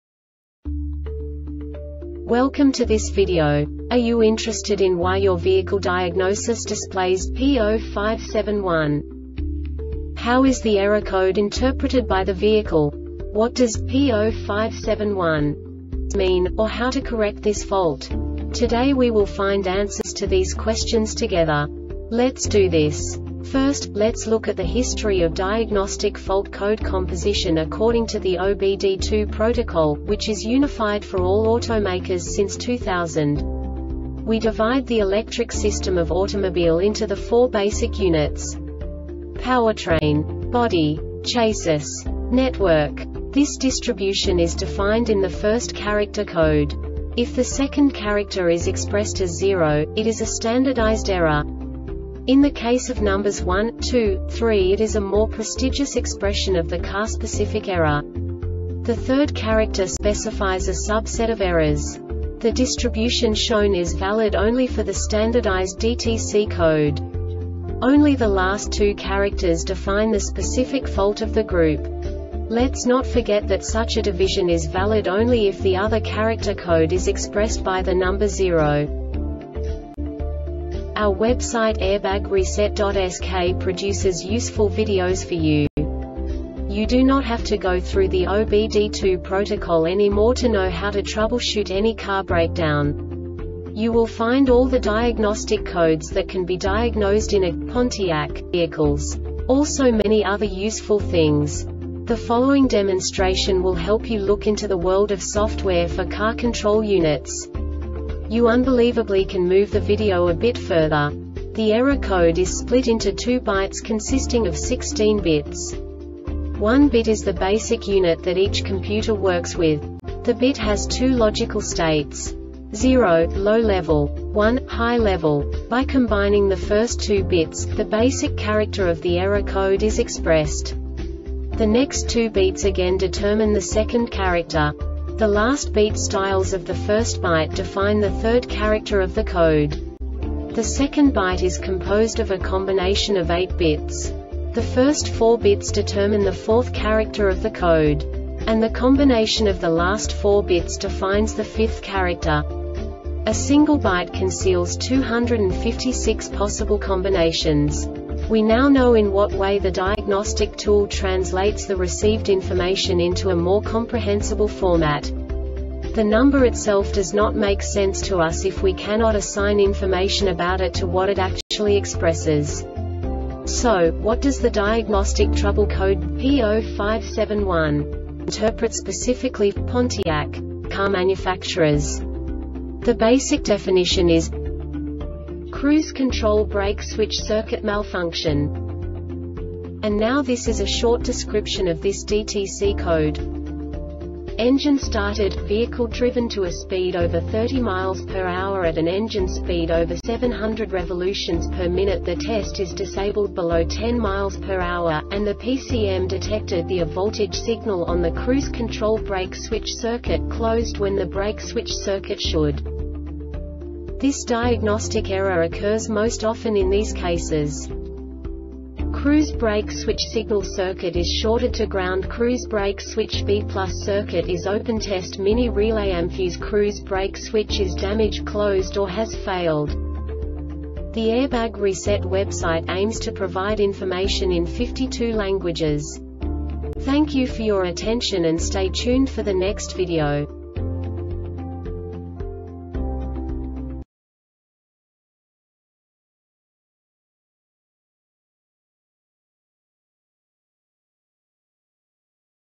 DTC Pontiac P0571 Short Explanation
Contents:
0:21 Basic DTC analysis according to OBD2 protocol standard.
1:48 Insight into programming
2:58 A diagnostically understandable description of the Diagnostic Trouble Code
3:05 Basic error definition

Make:
Pontiac
Code:
P0571
Definition:
Cruise Control Brake Switch Circuit Malfunction
Description:
Engine started,vehicle driven to a speed over 30 mph at an engine speed over 700 rpm (the test is disabled below 10 mph),and the PCM detected the a voltage signal on the Cruise Control Brake Switch circuit (closed) when the Brake Switch circuit should b
Cause:
Cruise brake switch signal circuit is shorted to groundCruise brake switch B+ circuit is open (test MINI Relay &amp;fuse)Cruise brake switch is damaged (closed) or has failed